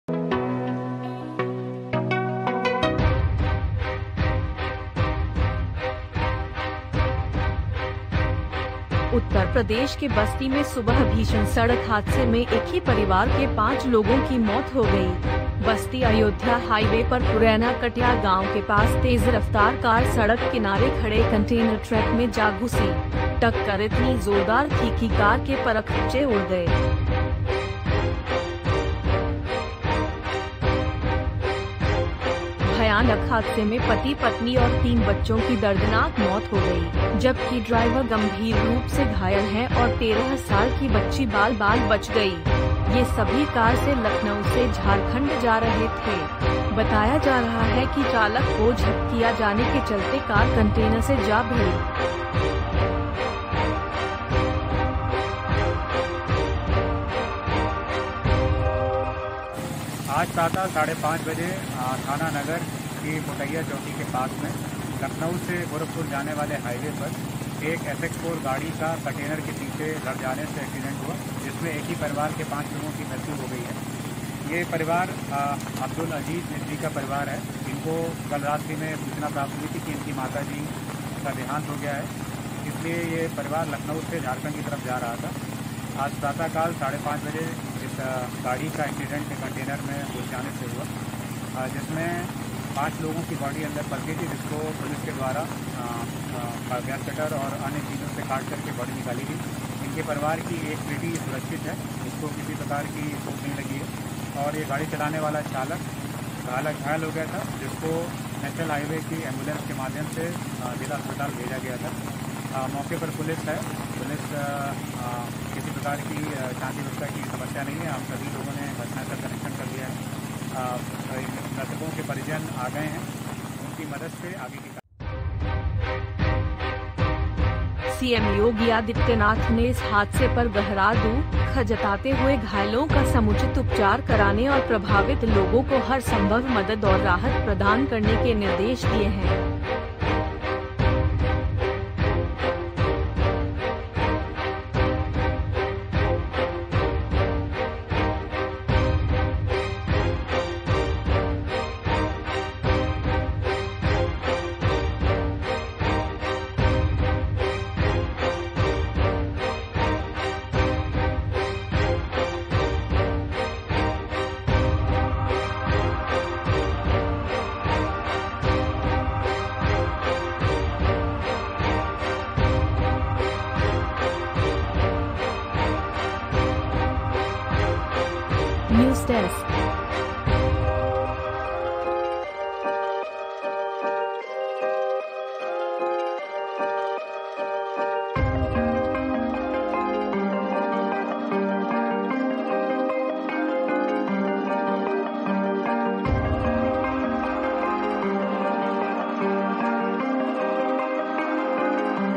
Jharkhand के एक परिवार से 5 लोगों की मौत, UP के Basti में कंटेनर से टकराई Car, हुआ भीषण हादसा
उत्तर प्रदेश के बस्ती में सुबह भीषण सड़क हादसे में एक ही परिवार के पांच लोगों की मौत हो गई। बस्ती अयोध्या हाइवे पर पुरैना कटया गांव के पास तेज़ रफ़्तार कार सड़क किनारे खड़े कंटेनर ट्रक में जा घुसी। टक्कर इतनी जोरदार थी कि कार के परख्च्चे उड़ गए।